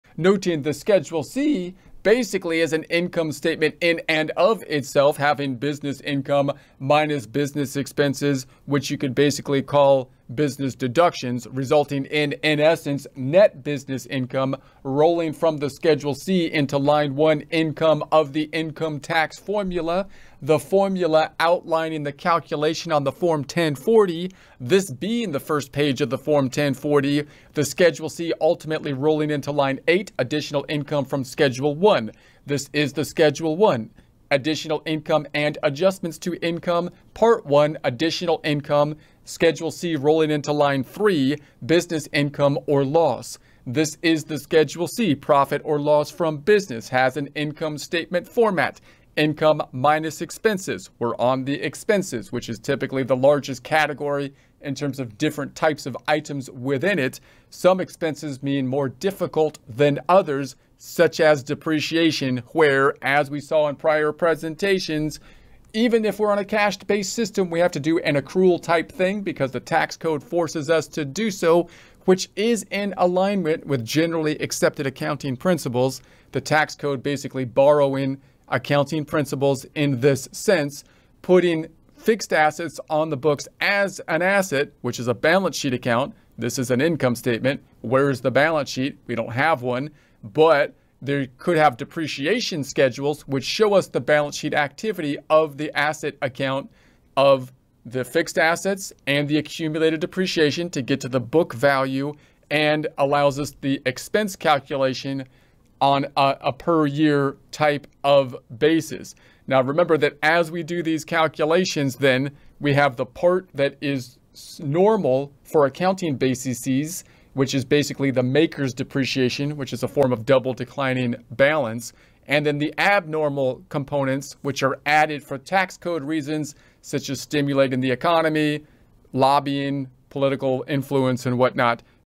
Depreciation Concepts Tax 2023-2024
"Dive into the dynamic world of depreciation concepts for the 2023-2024 tax year! Our short video elucidates key depreciation principles, helping you navigate tax obligations with clarity and confidence.